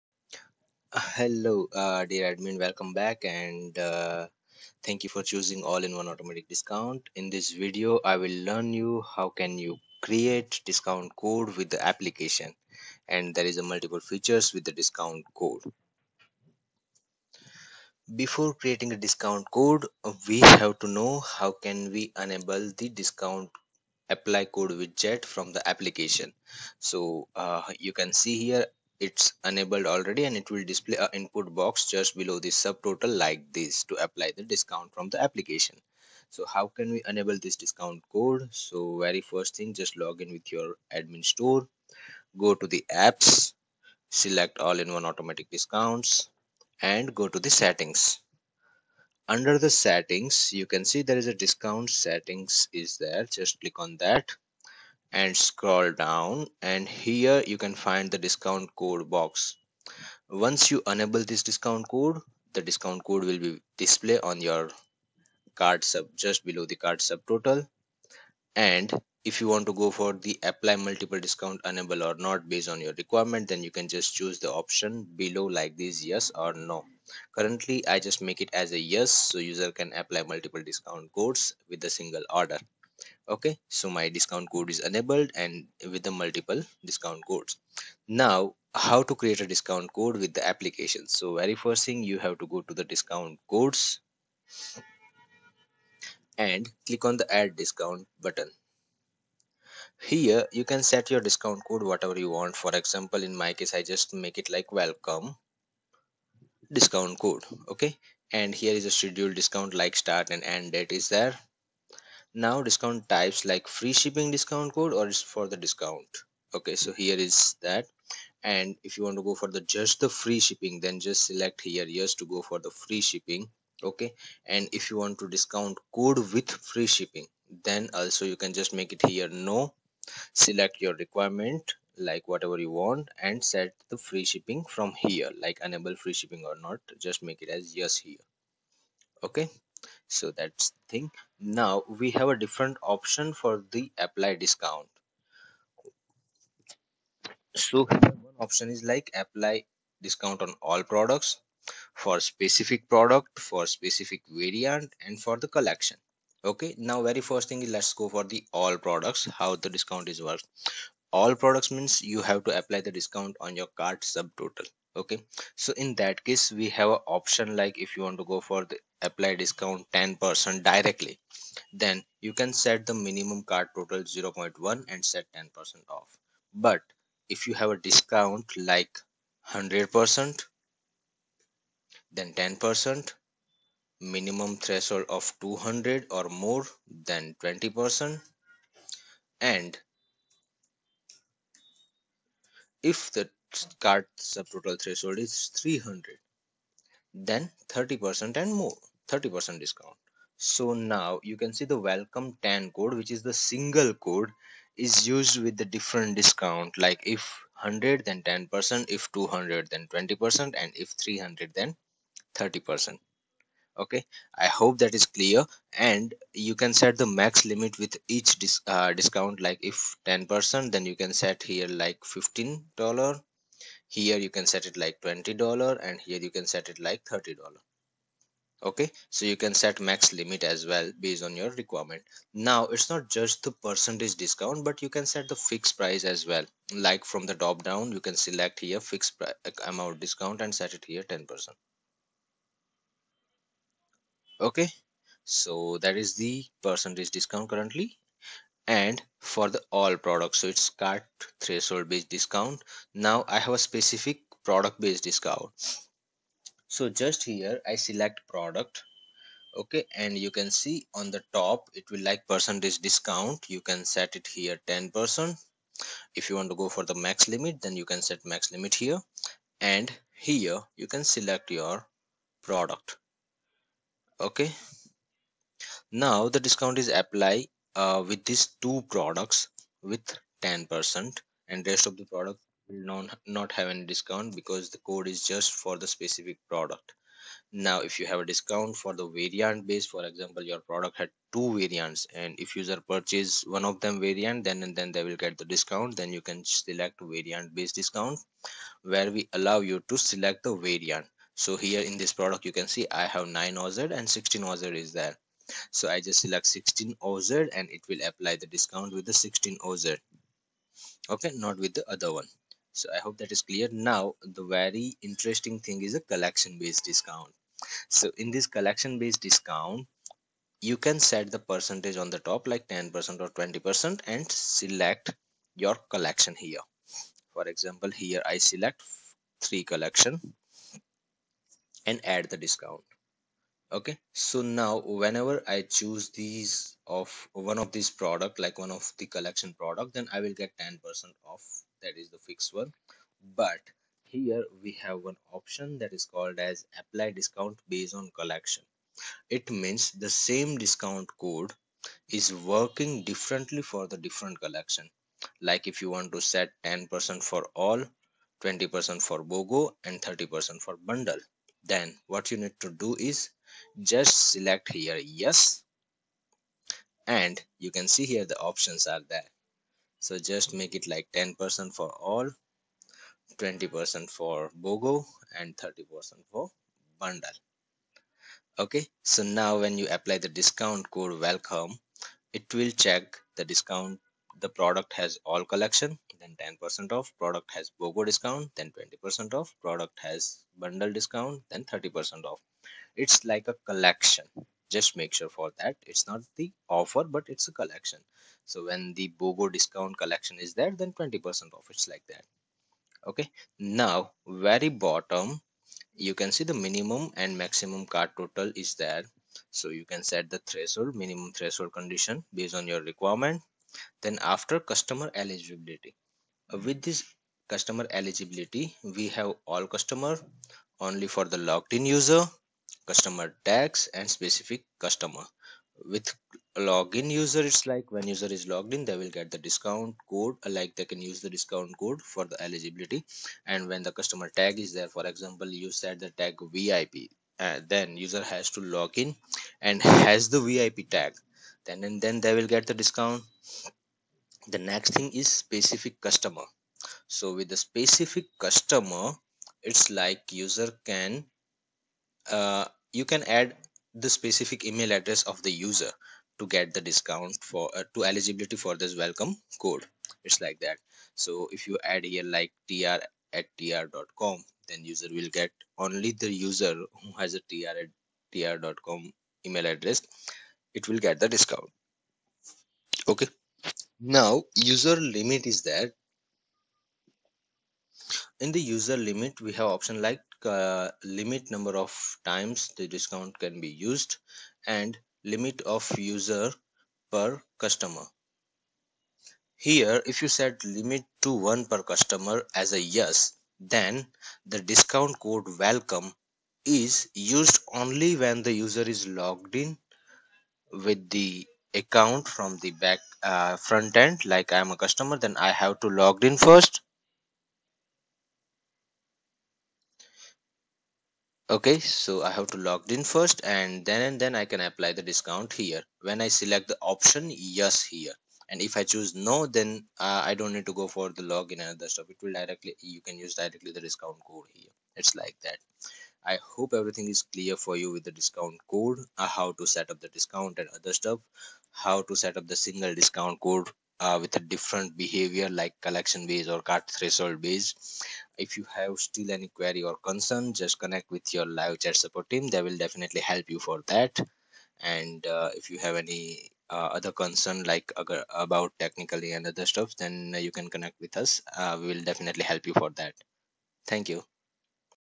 Single discount code different discount % Based on Cart Total or Collection | Automatic Discount app
Shopify discount code behavior is straightforward.
You can create one discount code for a percentage discount. It does not allow you to create a discount code that works differently based on cart subtotal or collection.

All In One Automatic Discounts allow you to setup the a discount code like this. A single discount code works as per your cart subtotal or collection.

For ex: WELCOME is the discount code
if cart total is
100+ then 10% off
200+ then 20% off
300+ then 30% off

Or based on collection like below
If collection X then 15% off
if collection Y then 20% off
If collection Z then 25% off